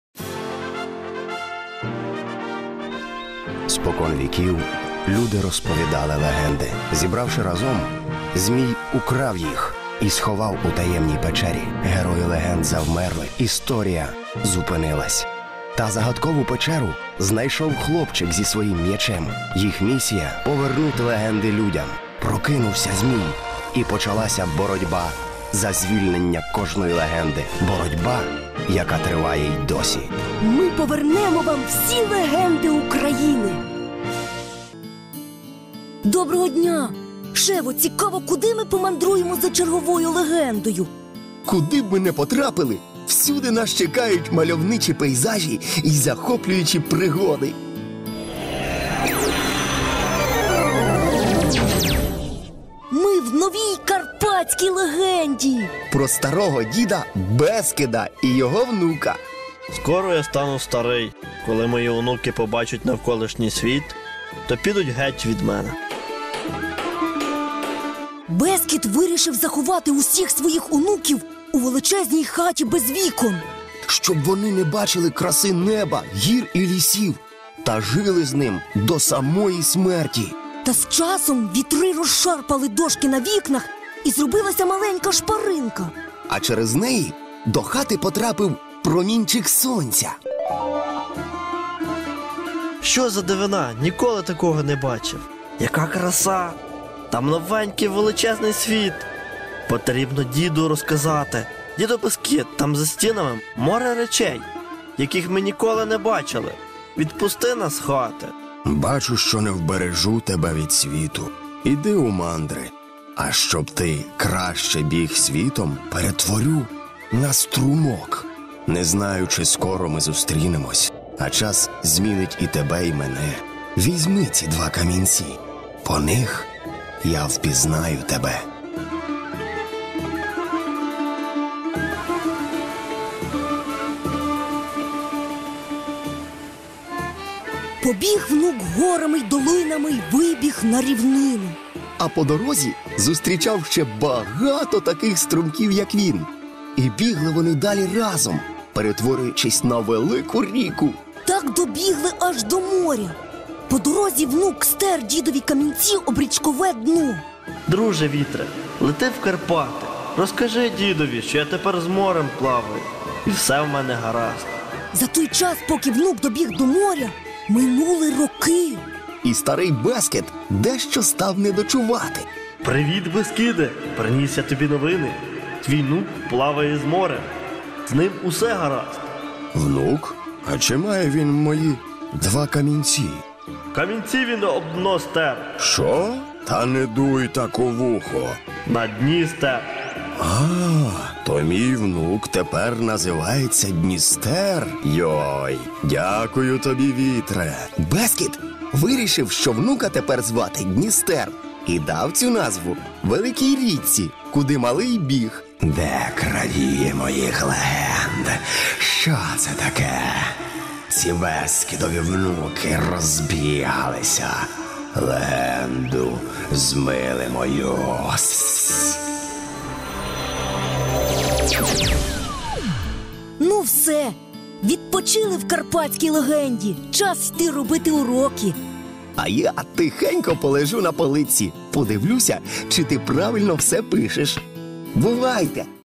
Кмітливий онук Бескида - Легенди України
Кмітливий онук Бескида

    Ще одна  неймовірна легенда про казкові українські Карпати, або як інакше називають наші гори - Бескиди. Точніше - про діда Бескида!
 Так от , - надумав якось сивий Бескид заховати своїх дітей та онуків, щоб на старості веселіше йому було , у хатині без вікон...Щоб не бачили вони ні земної, ні небесної краси... - а жили з дідом довіку.
Але якось, одного дня, пробився до хати крізь шпаринку промінчик сонця!
Як не важко було та мусив змиритись старий і відпустити нащадків на волю... А от,  що саме придумав Бескид , щоб один з його онуків швидше біг світом - довідуйтесь у нашій новій легенді!
Пізнавайте легендарну Україну разом з  нашими невтомними героями!